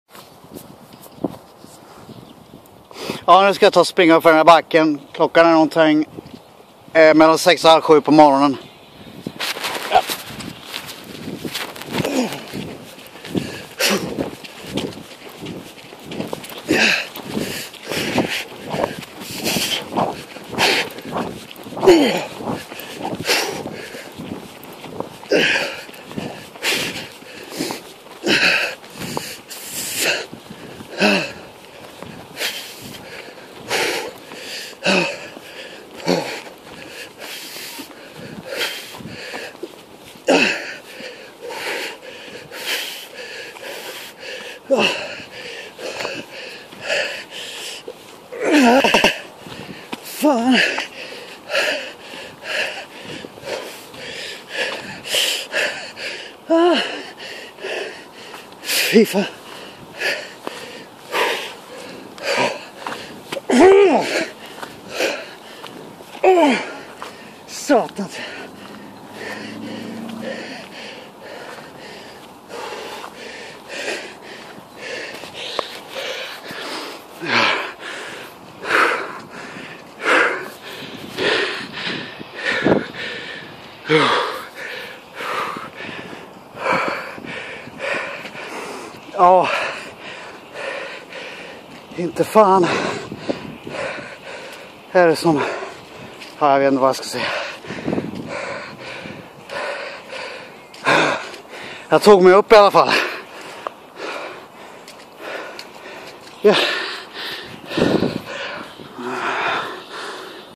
Sprang upp för vrinnevibacken lite efter kl 6:00 på morgonen....
Sprang upp för Vrinnevi slalombacken!

Nånting efter kl 6:00 på morgonen.
vidrigt jobbigt. Kunde knappt veta vad jag heter sådär dax och sluddrade nästan haha!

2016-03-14